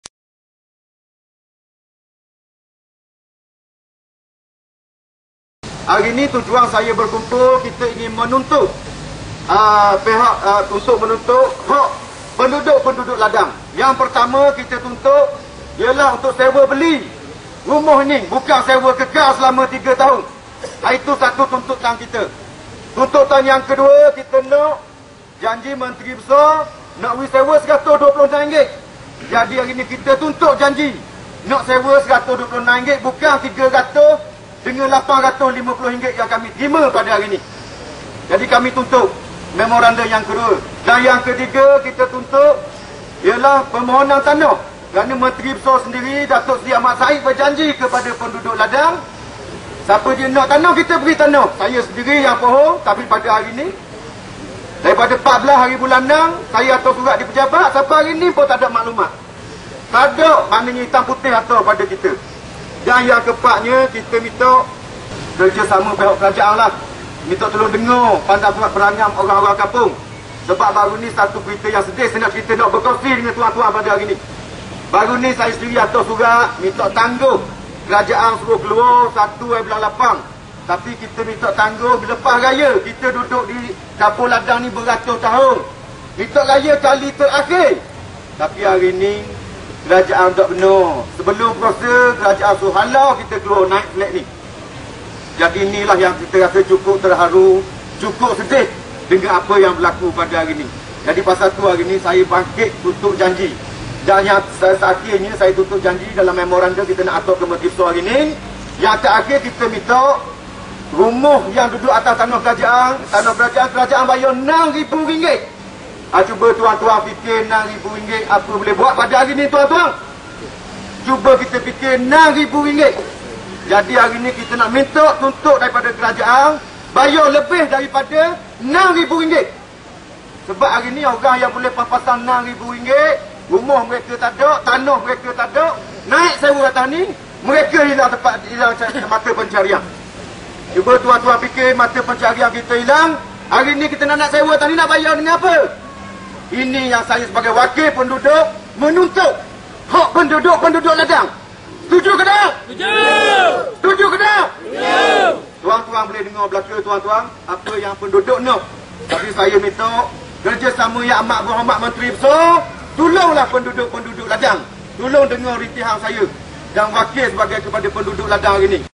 Ladang.net.mp4
Protes penduduk Kg. Ladang, Kuala Terengganu terhadap kerajaan berhubung janji-janji pengambilan tanah mereka.